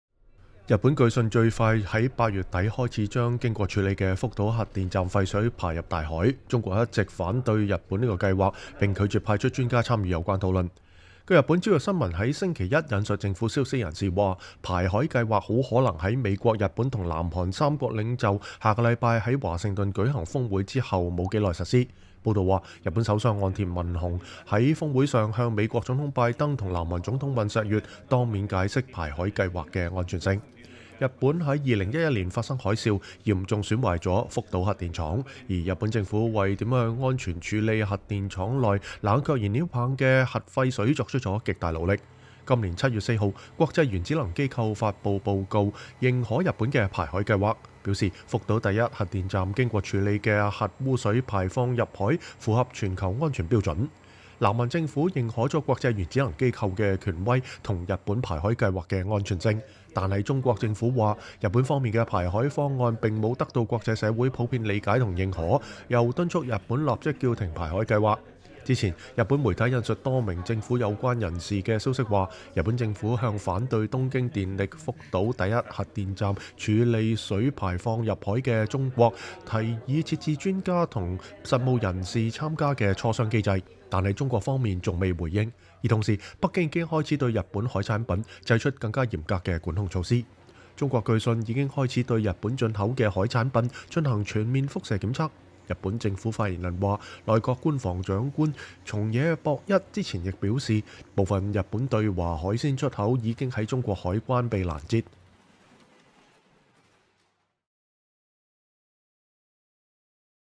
報導：日本將在美日韓下周峰會後開始將經處理的核廢水排入大海
日本據信計劃最快在8月底開始將經處理的福島核電站廢水排入大海。中國一直在竭力反對日本這一計劃，並拒絕派出專家參與有關的討論。